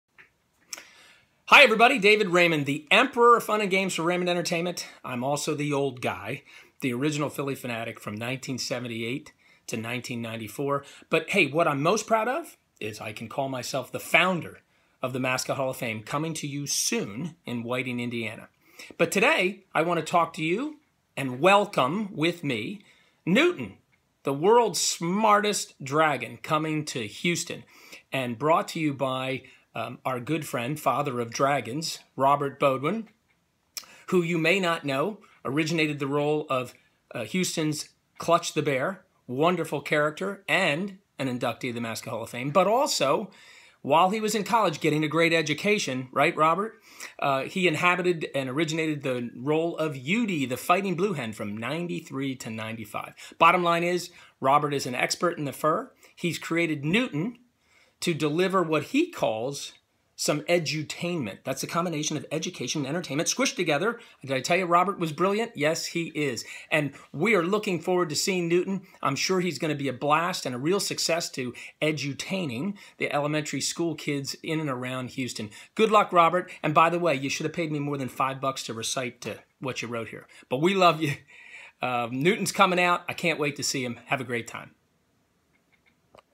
Dave Raymond Original Phillie Phanatic 1978-1993 on NEWTON "Houston's Smartest Dragon"
The Mascot Hall of Fame Founder and performer trailblazing performer talks DRAGON!